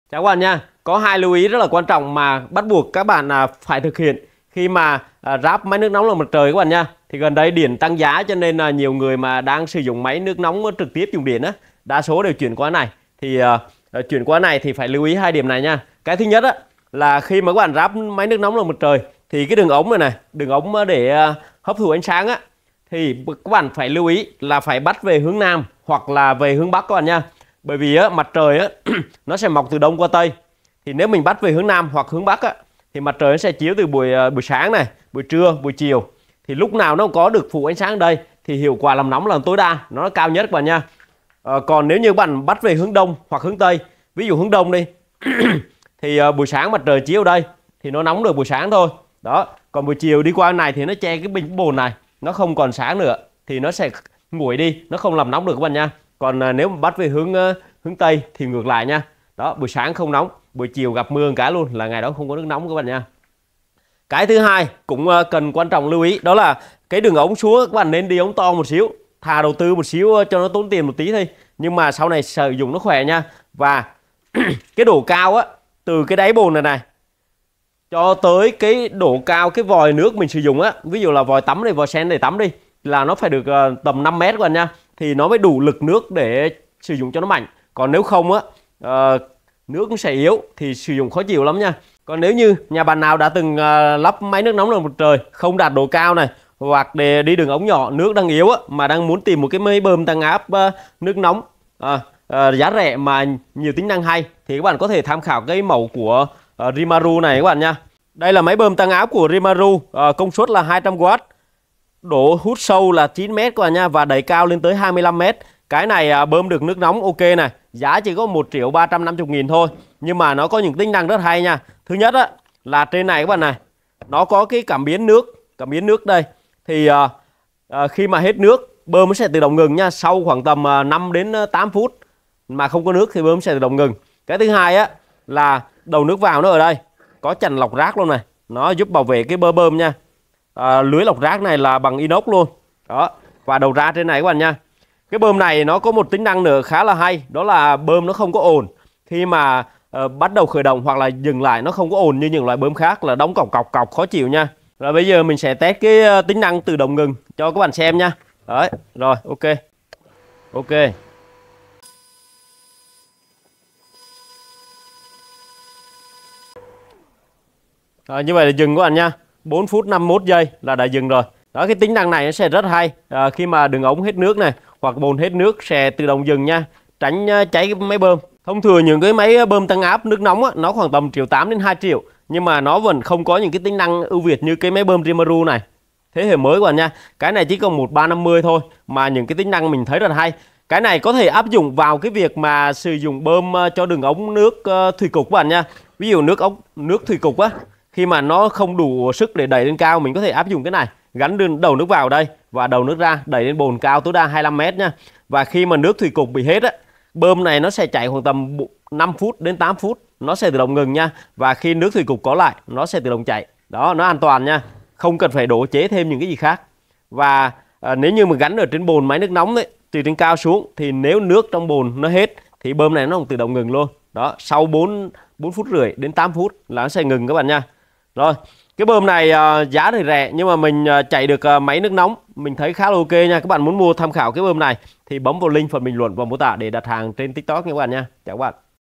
top 2 lưu ý cần phải thực hiện khi lắp máy nước nóng năng lượng mặt trời, máy bơm tăng áp nước nóng
các bạn bấm link này để mua máy bơm tăng áp nước nóng rymaru trên tiktok nhé.
top 2 lưu ý cần phải thực hiện khi lắp máy nước nóng năng lượng mặt trời, máy bơm tăng áp nước nóng rymaru này sẽ là thứ mà rất nhiều nhà đang cần vì những tính năng hay và cần thiết cho mọi nhà.